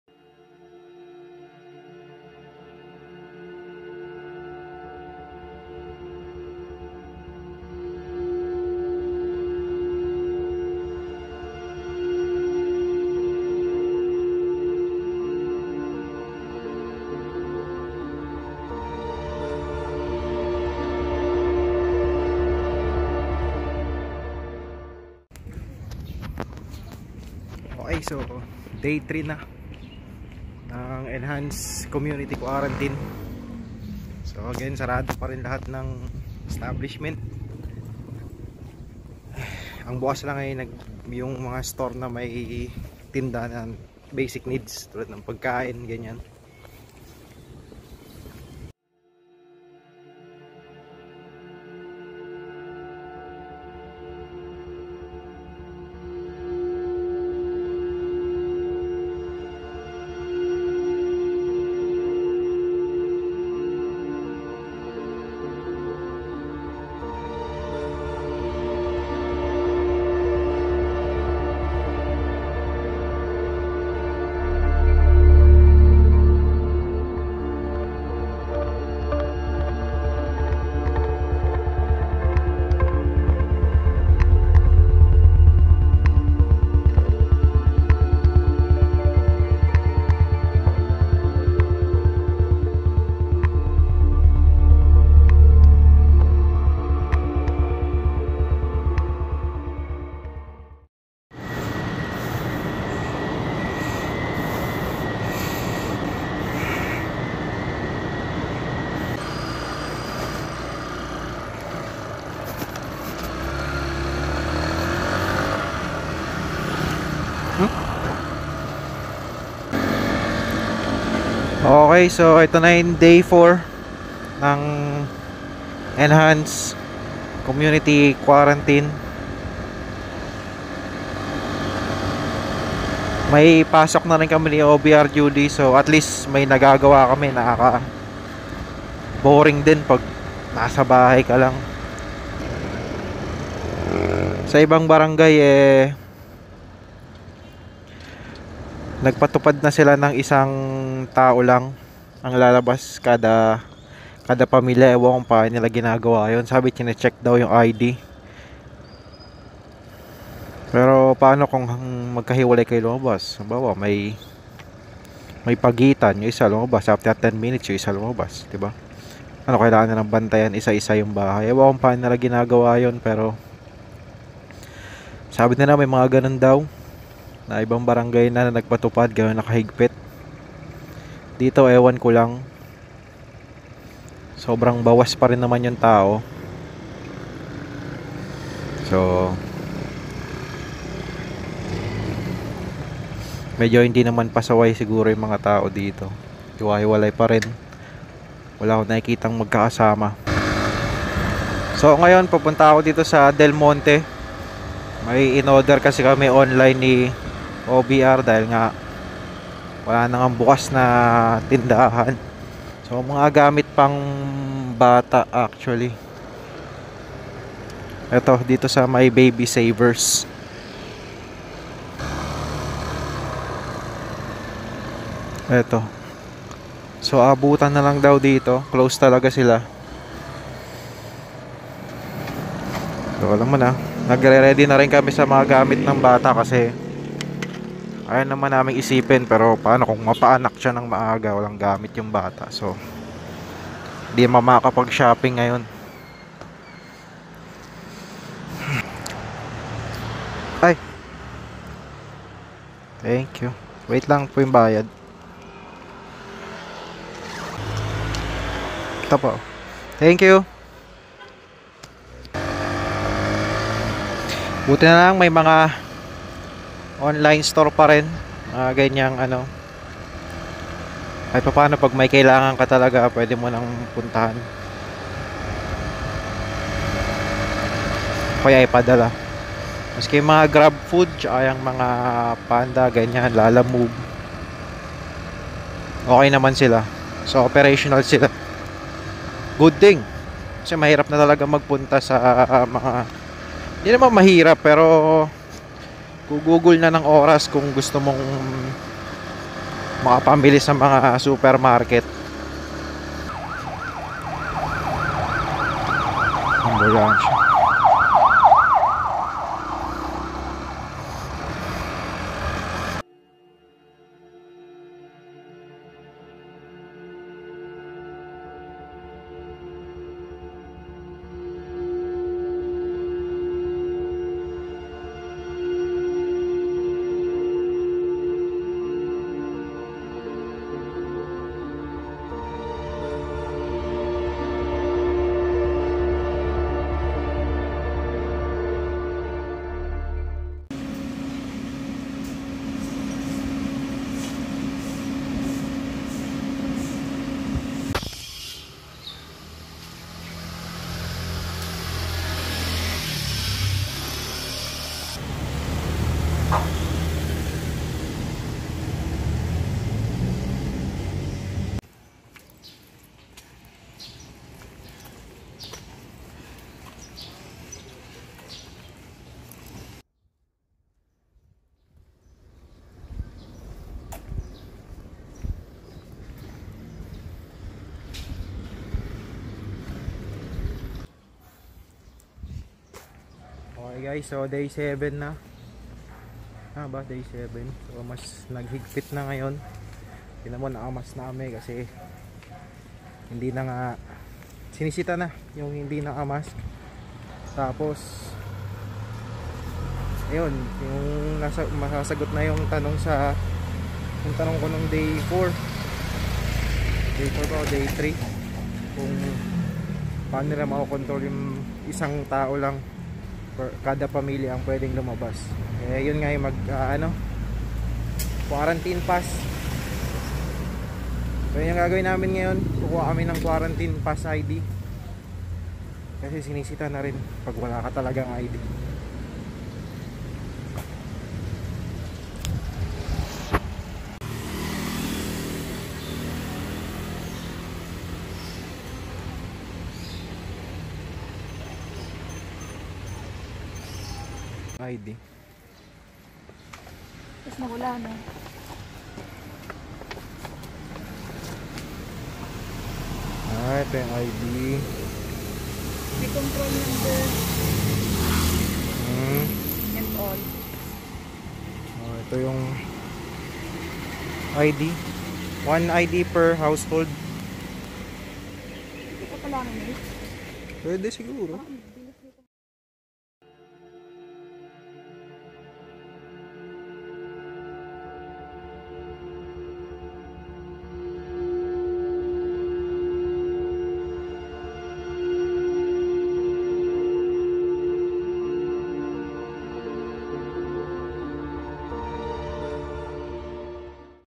PNJS: COVID-19 | ENHANCED COMMUNITY QUARANTINE | DAY 3,4 AND 7
It's getting stricter and stricter everyday. This time you cannot go out unless you have a quarantine pass and only one ID is given on each premise.